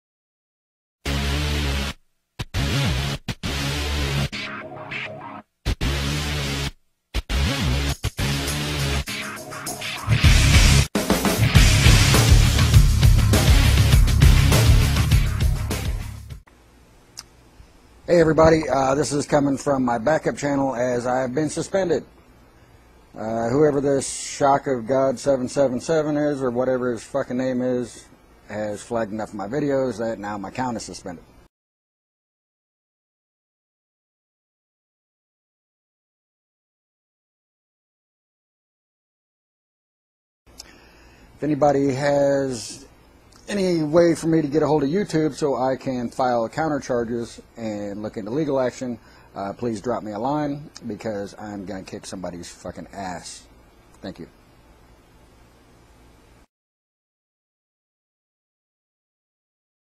Voodoosixxx falsely flagged -mirror-
A user named ShockofGod777 has falsely flagged many of Voodosixxx's vids and got him suspended, so he had to open a new account. It is likely that SoG777 may just be a troll and NOT the infamous fundie dolt ShockofGod using a different name.

edit 6/23/10: It turned out that the person behind SoG777 wasn't ShockofGod after all. I was dissapointed to learn that it was user AllianceofDemons behind it. He has actually done many great videos exposing how much of an asshole the real SoG is, and this has caused me to lose some respect for him. He has said that this was only meant as a prank and that he got carried away. Link to his apology